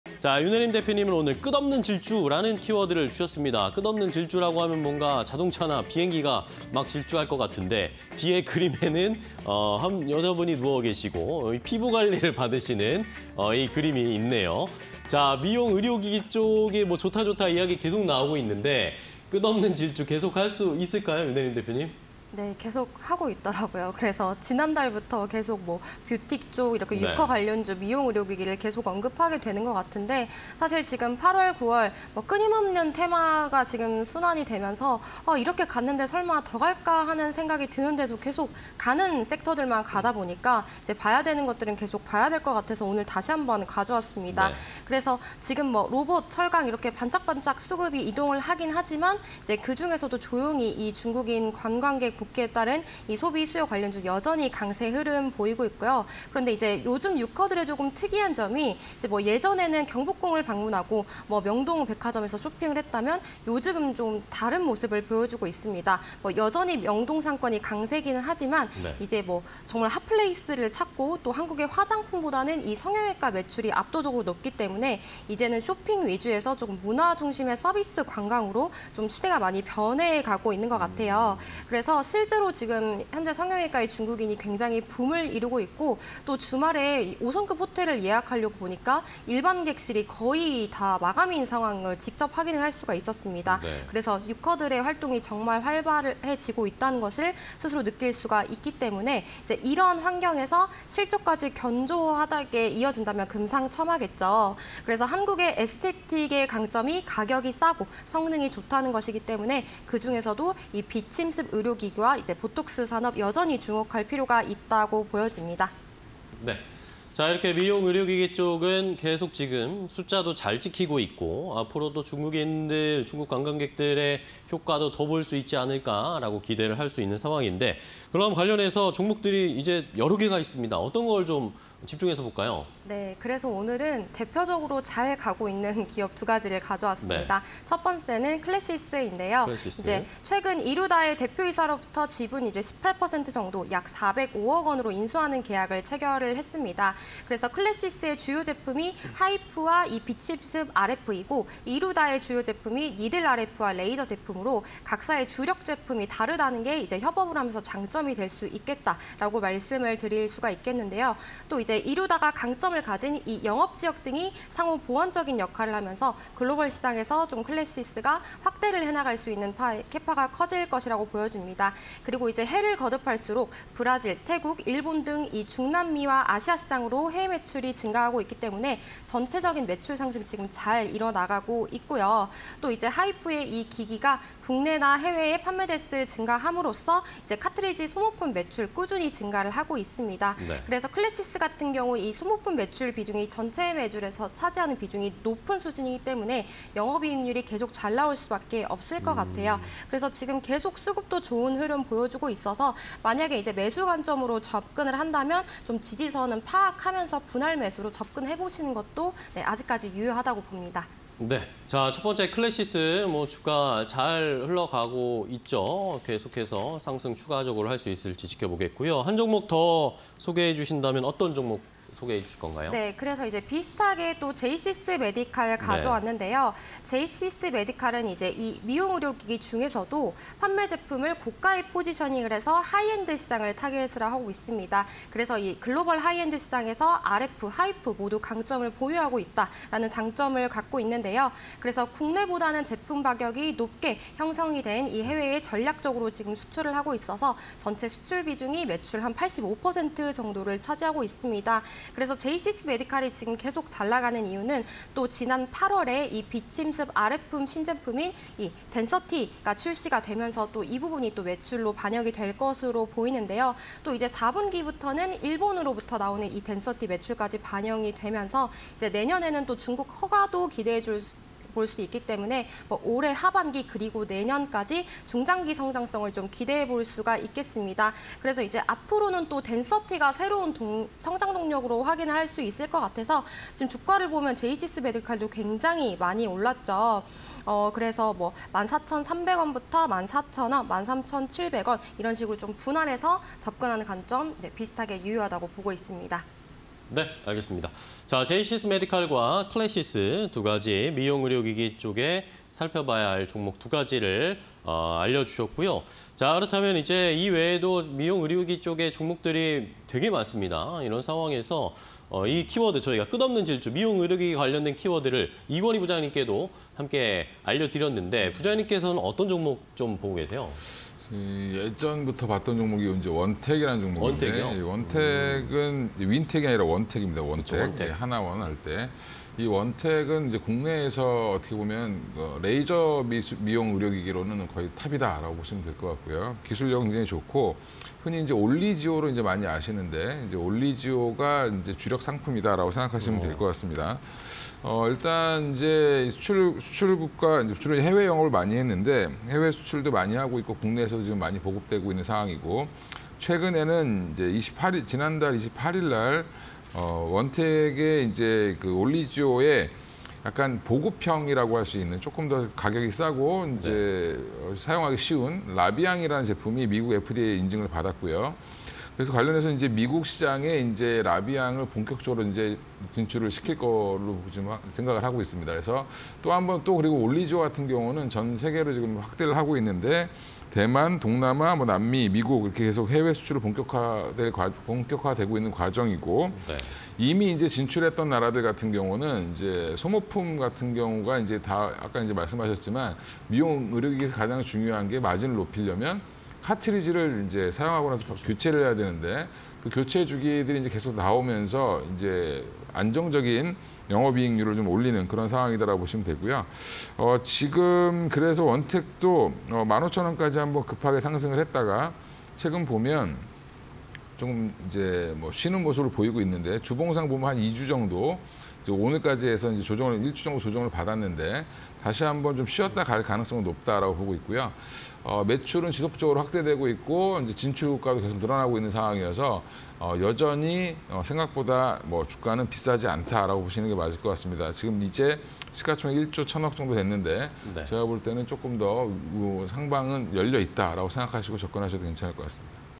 클래시스(214150), 제이시스메디칼(287410), 원텍(336570)… 미용의료기기_키워드 배틀 (20230911)
- 중국인 소비 관련주 강세 지속
- 문화 중심의 서비스 관광으로 변화
- 성형외과 '유커' 붐·가성비 좋은 韓 에스테틱 선호
- 비침습 의료기기 및 보톡스, 지속적 주목

클래시스(214150)
- 최근 '이루다'로부터 지분 18% 405억 원 인수
- 중남미ㆍ아시아 시장 매출 상승세
- 소모품 매출 증가, 수익성 개선

제이시스메디칼(287410)
- 판매 제품, 고가 포지셔닝
- 하이엔드 시장 RFㆍHIFU 모두 강점
- 신제품 '덴서티' 새로운 성장 동력
- 내년 '포텐자' 중국 진출 기대

원텍(336570)
- '올리지오' 수출국 확대ㆍ영업 레버리지 효과 본격화
- '올리지오', 대만ㆍ동남아ㆍ미국 등 해외 수출 본격화
- 소모품 매출 비중 상승… 이익 고성장 사이클 진입